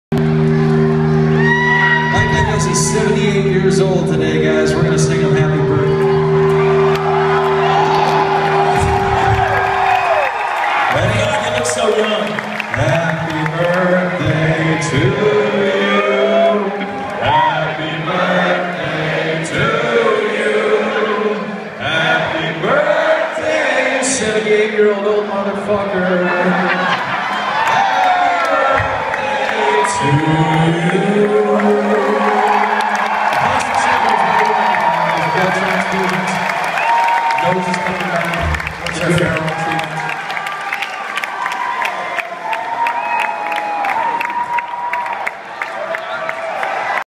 Happy Birthday, Mike Inez!
Bassist for Alice in Chains celebrates his birthday in Roanoke, Virginia. 5/14/2014